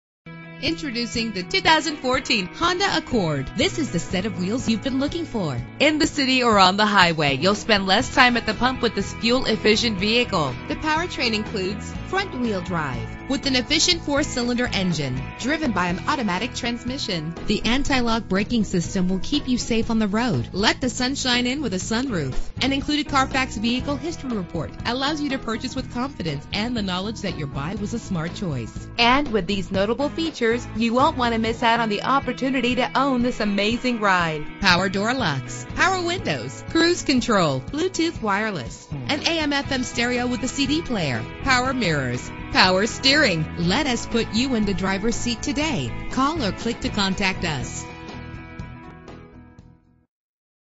2014 Honda Accord - Chattanooga TN
Year: 2014
Make: Honda
Model: Accord
Trim: EX
Engine: 2.4 liter inline 4 cylinder 16 valve GDI DOHC
Transmission: Automatic CVT
Color: Champagne Frost Pearl
Mileage: 12
Address:
2135 Chapman Road